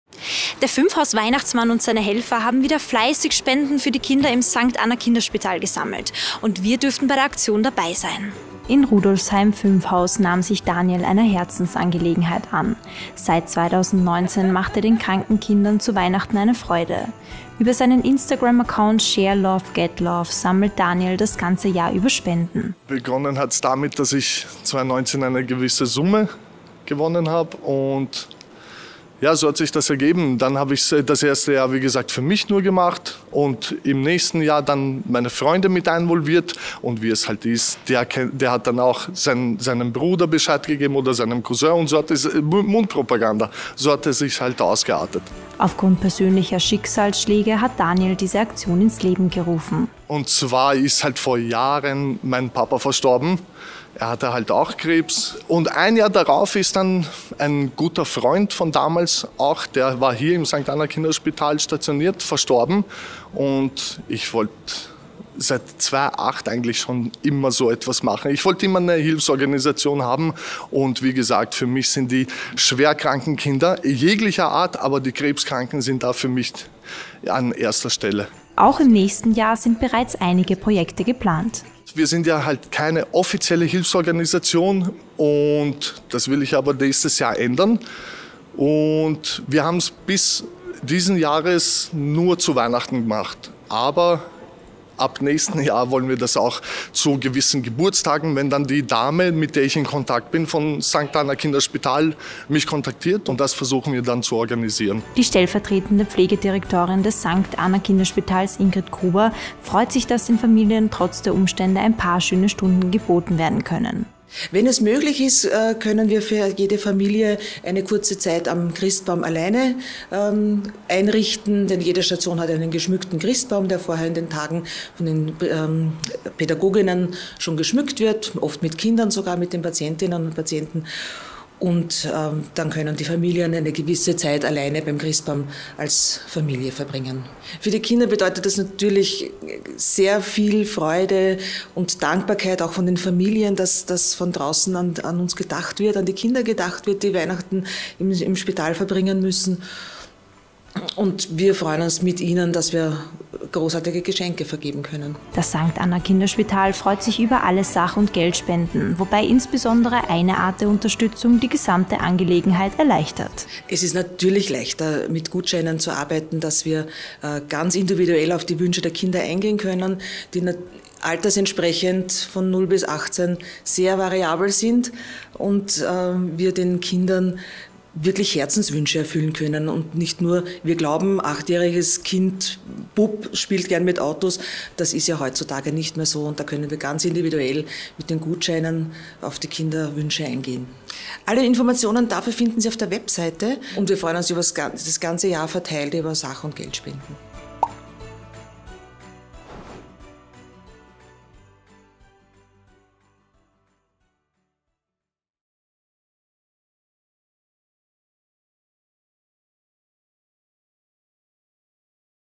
Fünfhaus-Santa besucht das St. Anna Kinderspital
Es gibt ihn also doch! Am Mittwoch freute sich das St. Anna Kinderspital auf einen besonderen Gast. Fünfhaus-Santa Daniel machte den Kids eine Freude.